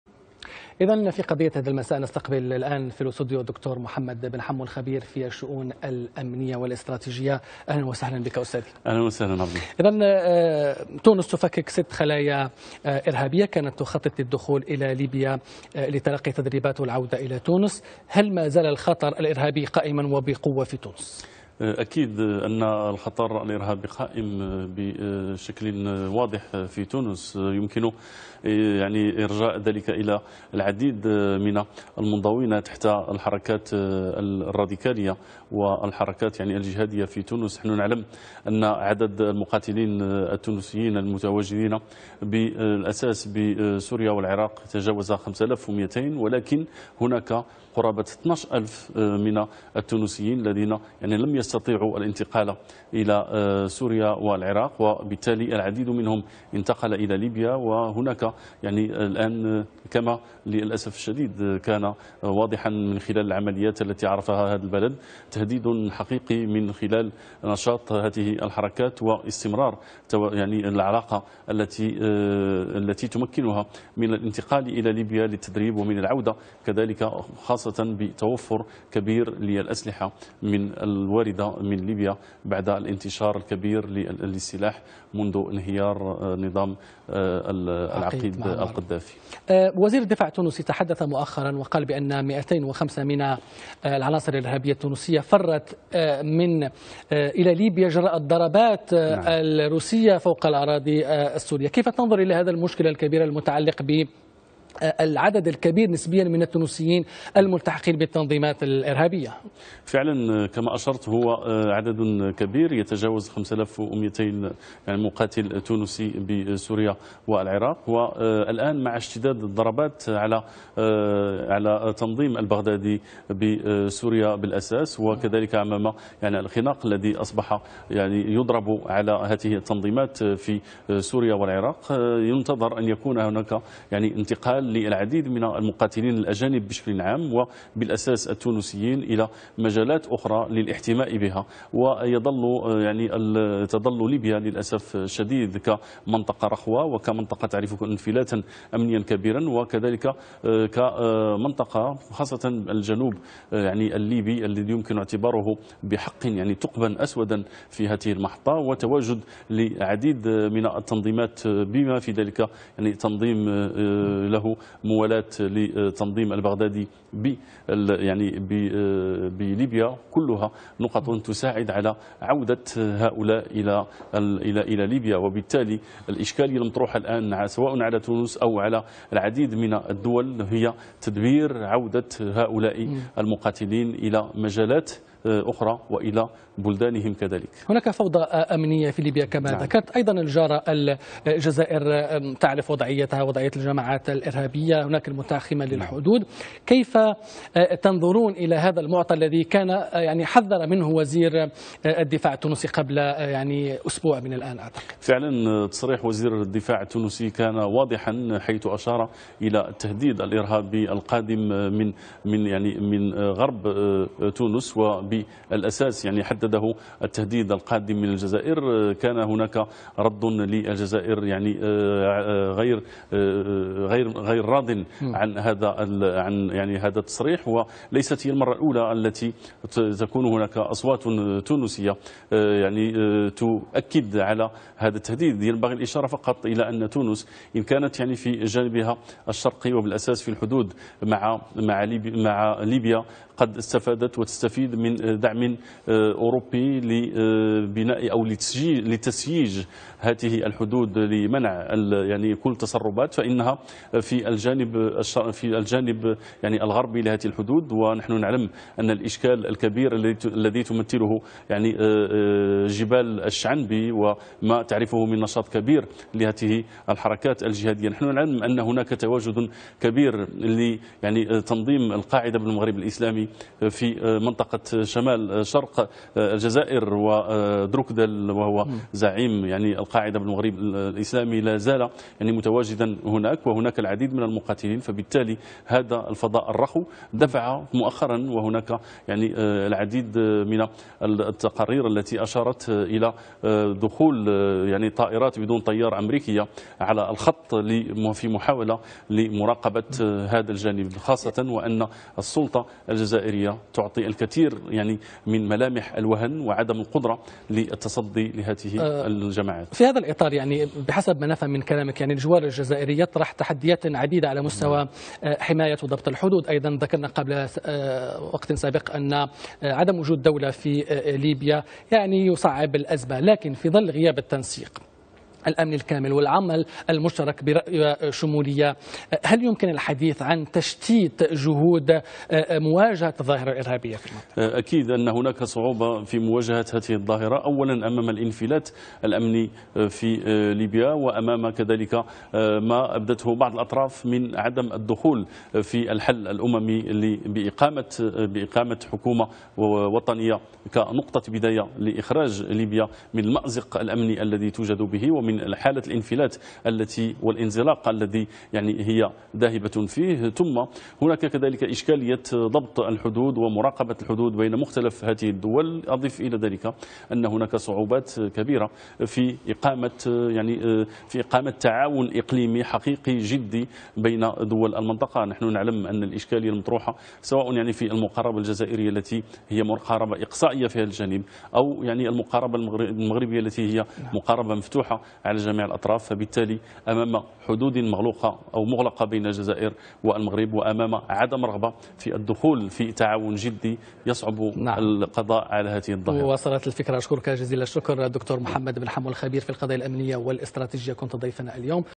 الخطر الإرهابي على تونس وعمل الأجهزة الأمنية للحد منه
يناقش الدكتور محمد بنحمو الخبير في الشؤون الأمنية، الخطر الإرهابي الذي يهدد تونس، وعمل الأجهزة الأمنية التونسية والتعاون بين مختلف دول المنطقة، وسبل الحد من تسلل الإرهابين من الحدود الليبية.